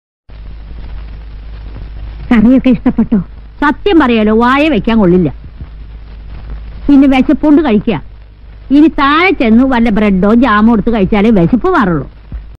കറിയൊക്കെ ഇഷ്ടപ്പെട്ടോ? YouTube Shorts - Nandana Chandran, Icheri, Sreekandapuram. GHSS Nedungome
കറിയൊക്കെ ഇഷ്ടപ്പെട്ടോ? YouTube Shorts - Nandana Chandran, Icheri, Sreekandapuram. GHSS Nedungome, ചങ്ങാതിക്കൂട്ടം Arts

Symphony Music ലെ ഗായികയും ചങ്ങാതിക്കൂട്ടം ആർട്സിലെ മൾട്ടി ടാലെന്റ്റ് ആർട്ടിസ്റ്റും Nedungome GHSS ലെ വിദ്യാർത്ഥിയും ശ്രീകണ്ഠപുരം ഐച്ചേരി സ്വദേശിനിയുമായ നന്ദന ചന്ദ്രൻ അവതരിപ്പിച്ച Dubsmash Comedy YouTube Shorts, Short Video